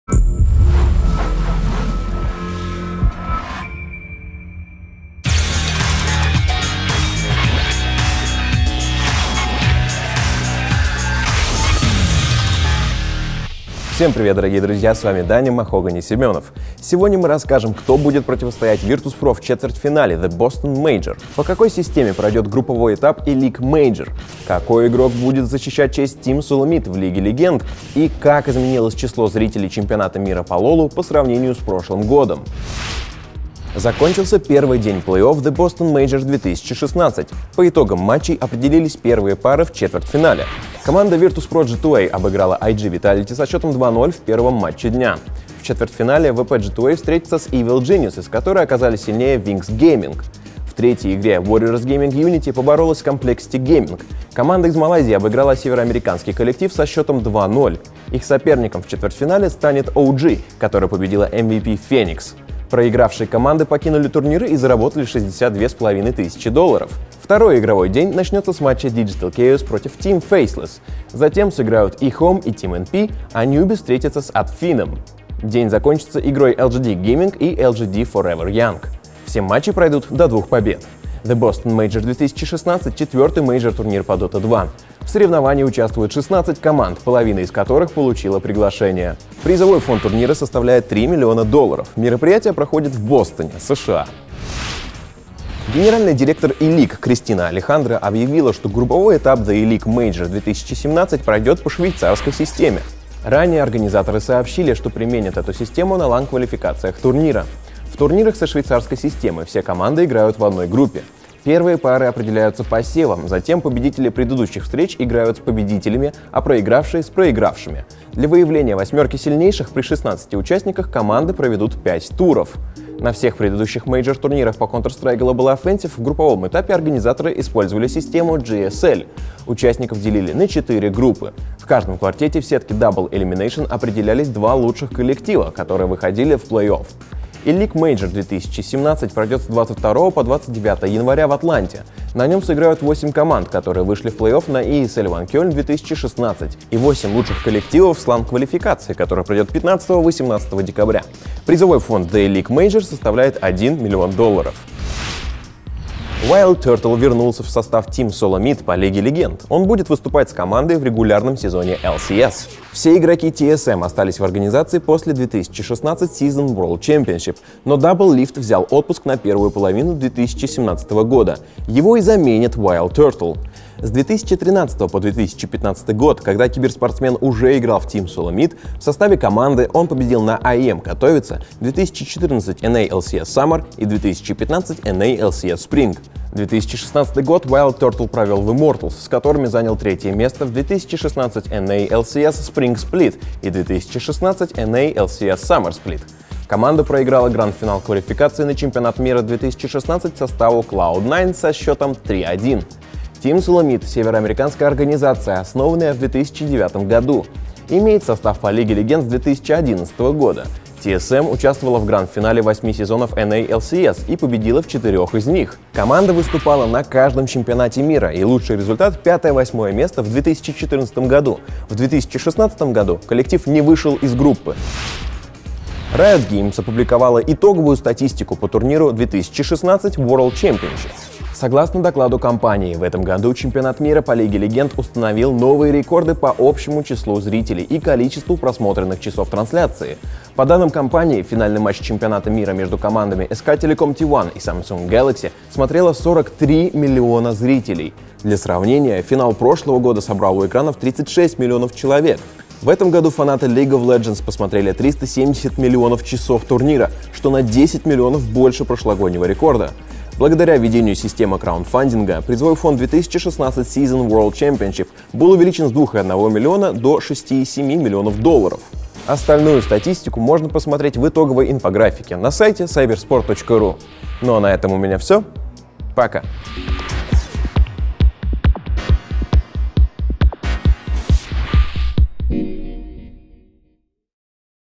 Новости: Virtus.pro vs Evil Geniuses на The Boston Major
В сегодняшнем выпуске новостей: подробности группового этапа ELEAGUE Major, итоги первого дня мейн-ивента The Boston Major и другие события на киберспортивной арене.